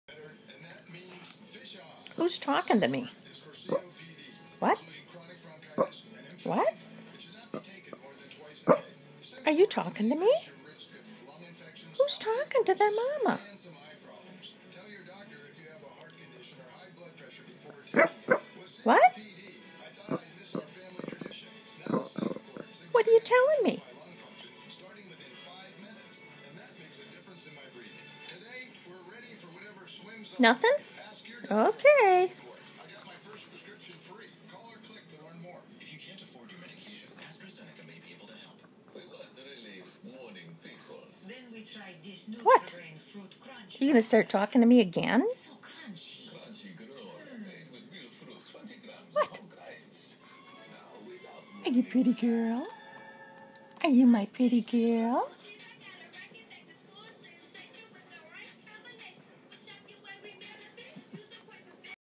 Annabelle talking to mama.
Shih-tzu Annabelle wants her mama's attention.